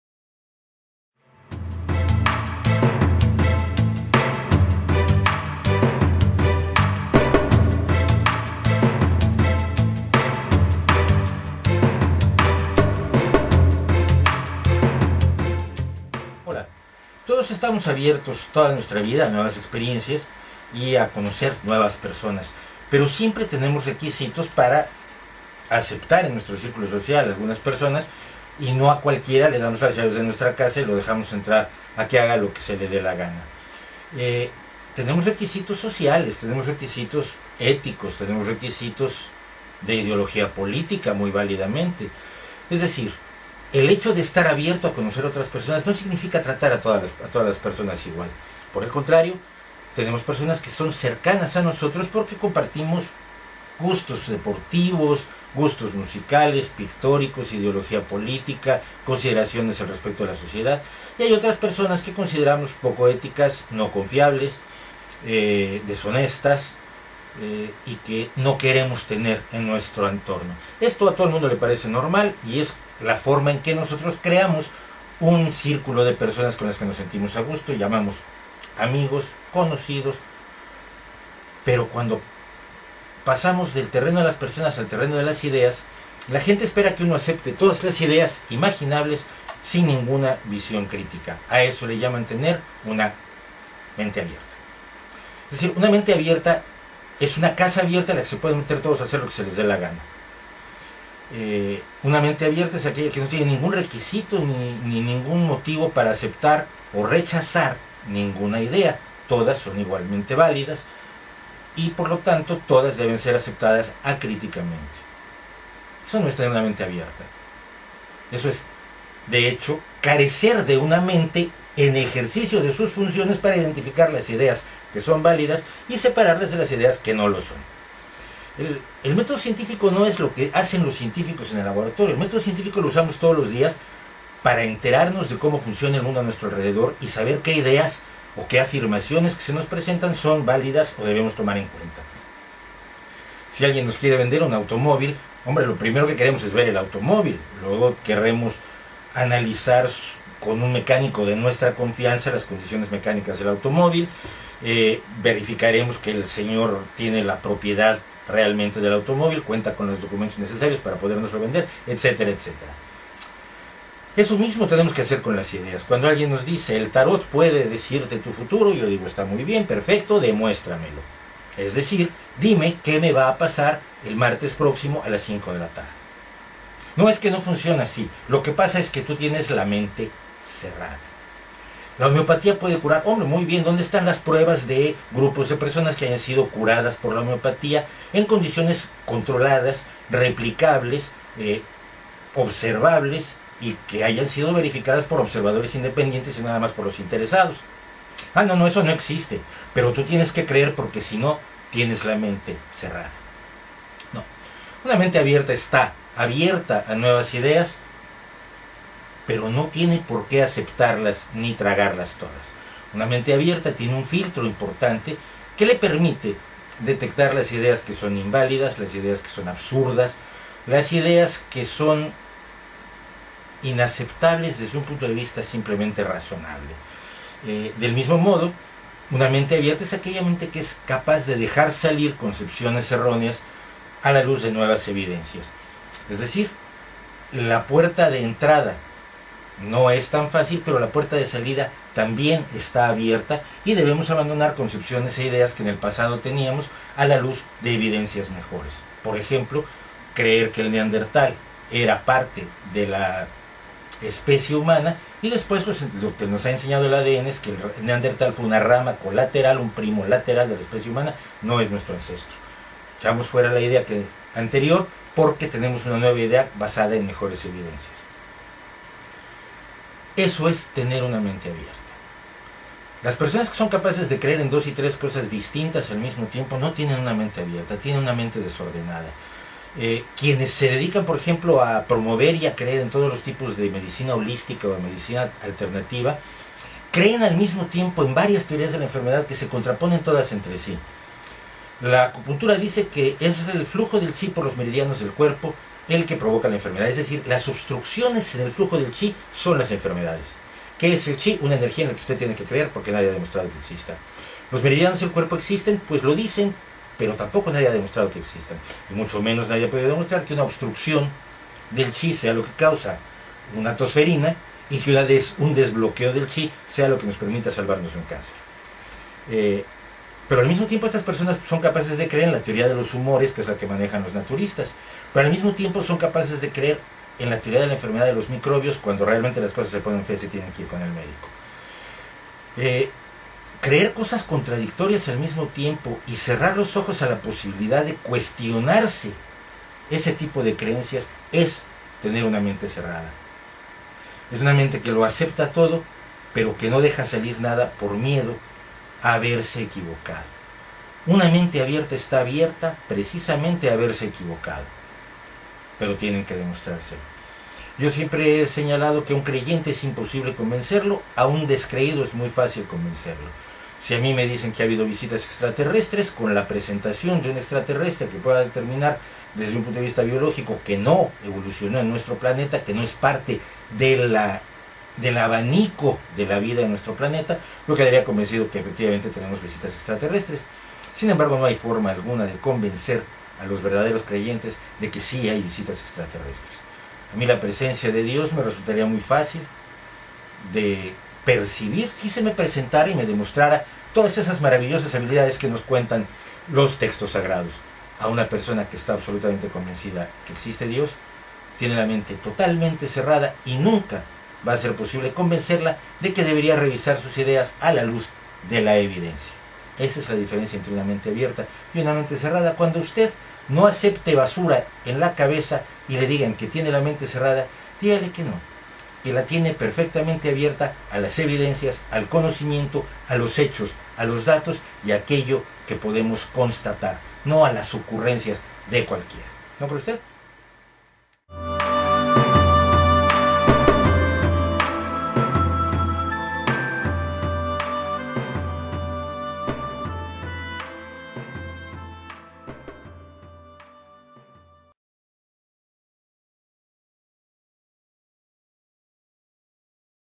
Del cielo a la tierra - Mente abierta... pero no a la basura
¿Qué es realmente tener una mente abierta o cerrada en cuestiones sobrenaturales?